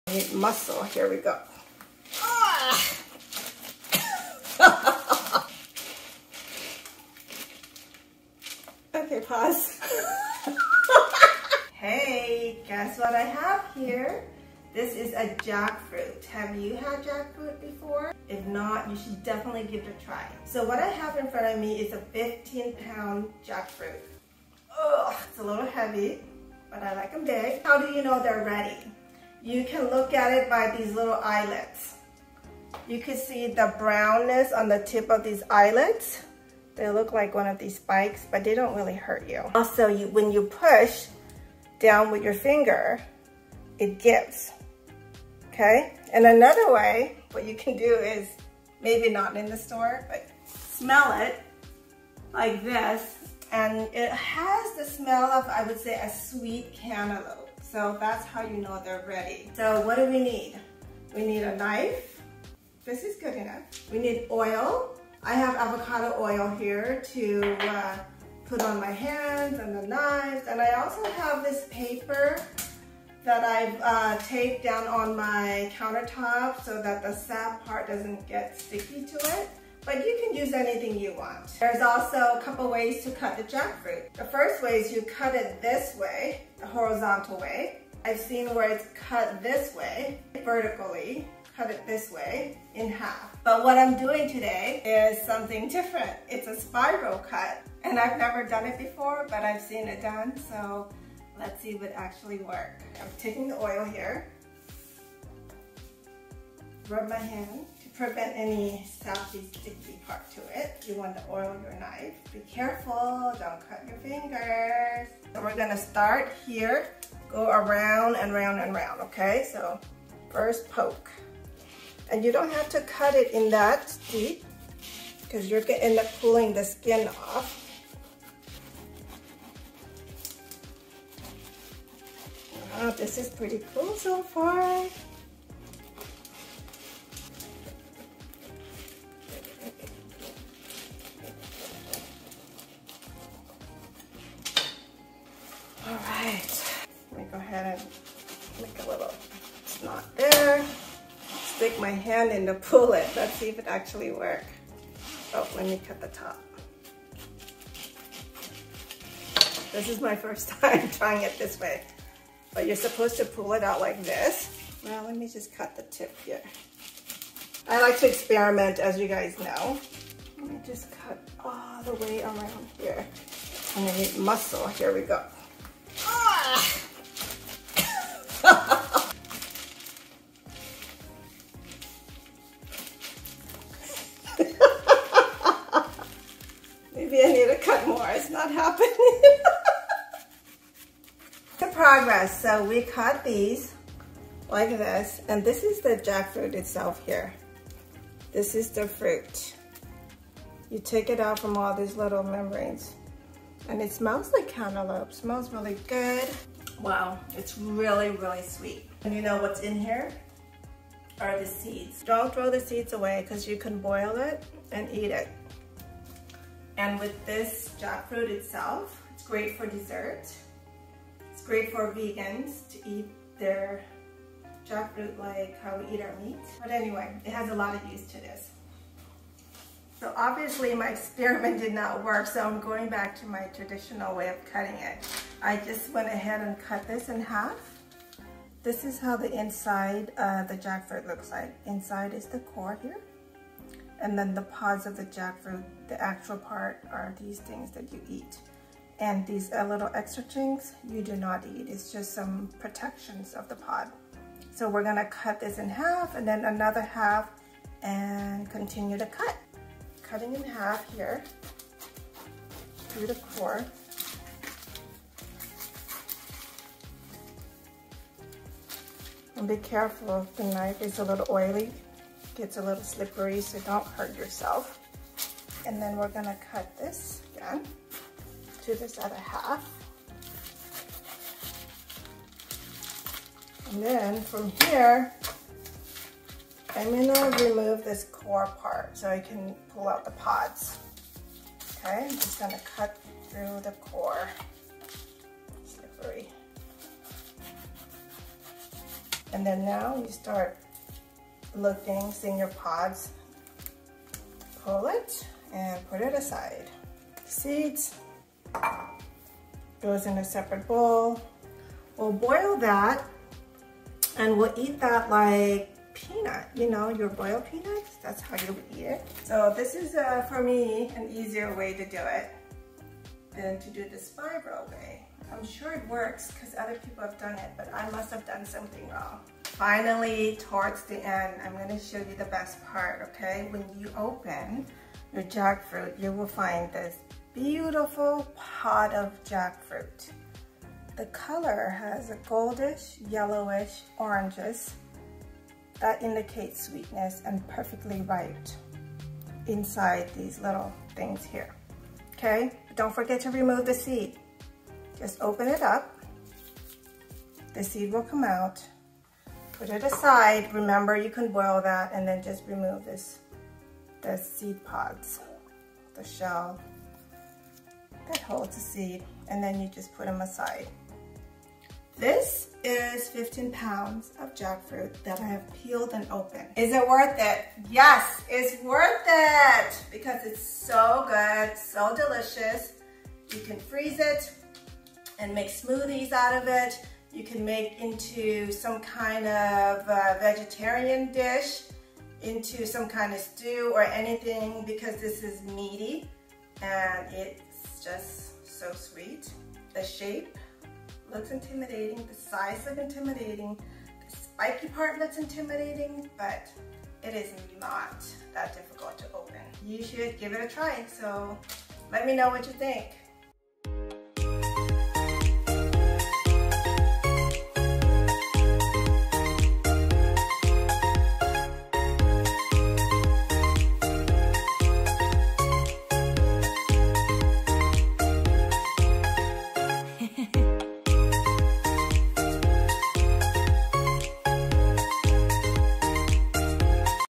Viral jackfruit cutting hack and the best way to cut it tutorial.
The viral jackfruit cutting hack. The best way to cut jackfruit tutorial.

Jackfruit is a tropical tree fruit grown in Asia, Africa and South America. Under its thick, bumpy rind is a stringy flesh that you can eat raw or cooked in a variety of dishes.

How to Cut Jackfruit: Step-by-Step Guide
Prepare a clean surface. Jackfruit has a sticky, white latex sap, so you'll want to handle it on a surface that is easy to clean. ...
Apply oil to your knife
Quarter the jackfruit.
Remove the core.
Remove the seeds and set them aside.
Remove the fruit pods.
Clean the fruit pods.